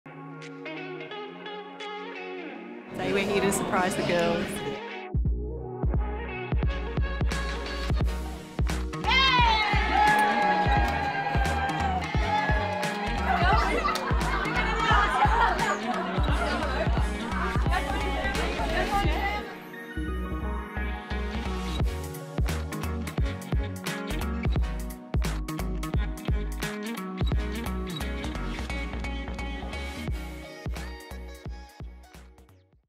Our families surprise us in Brisbane 🤫
Our families surprise us here in base camp in Brisbane.